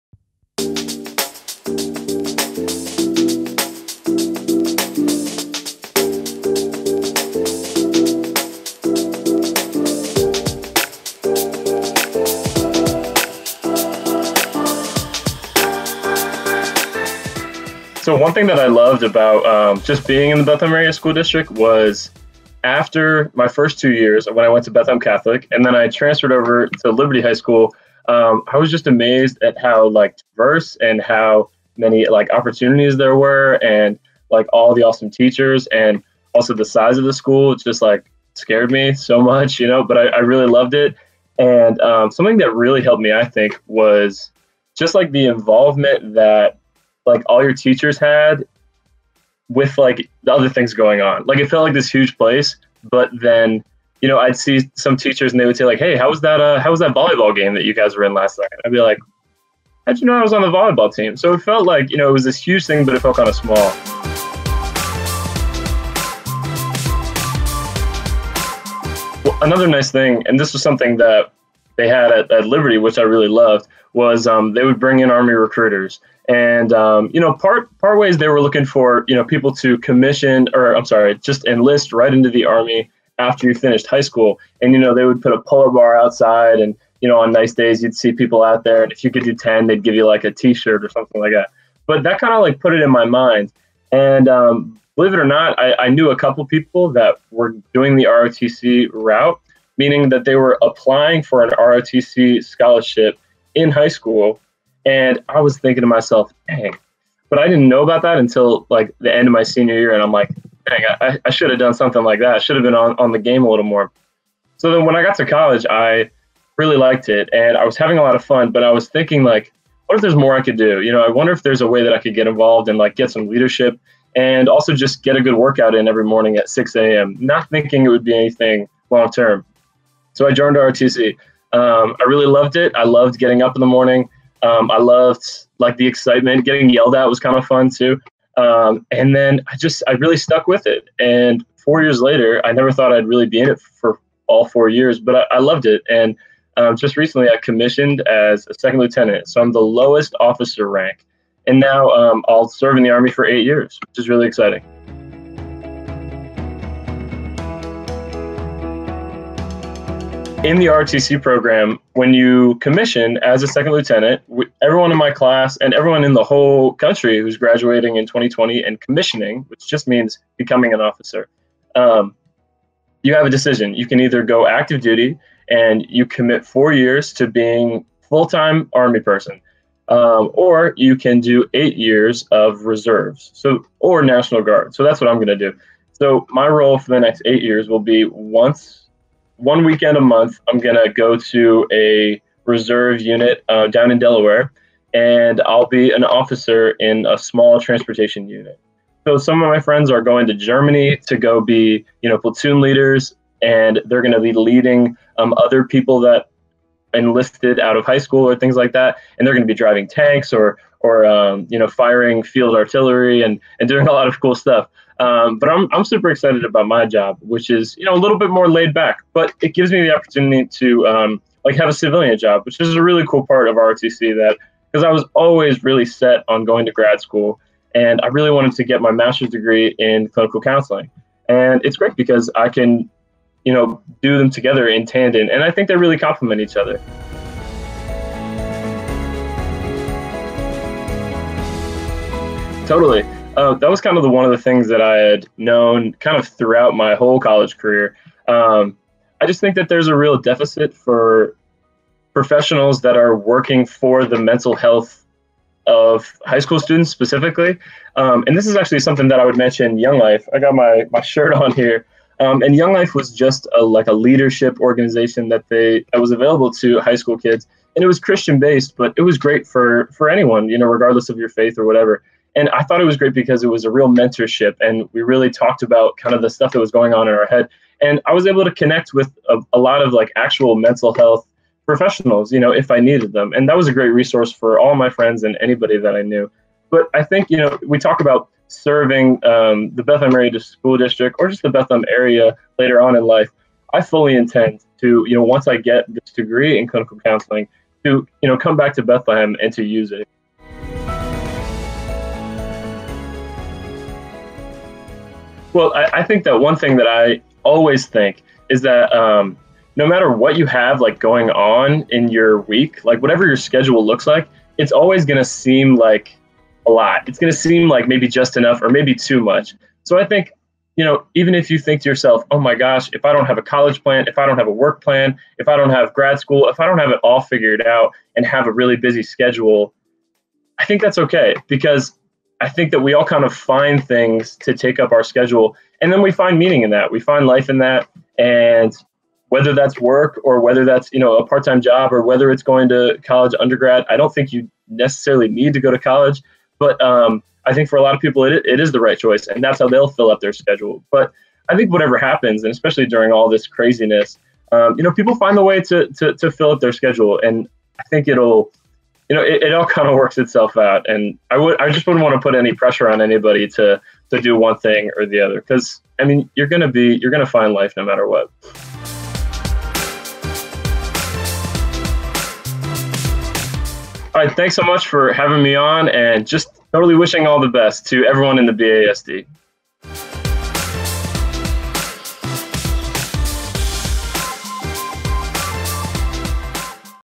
Built By Bethlehem: Grady Hackett
Welcome to our "Built by Bethlehem" interview series with BASD graduates.

First up is Grady Hackett, a Liberty High School alum who recently graduated from the University of Delaware with a bachelor of science in cognitive science.

Hackett was also recently commissioned as a 2nd Lieutenant in the Army Reserves.

Learn more about his future plans, his favorite parts of his BASD education, and more!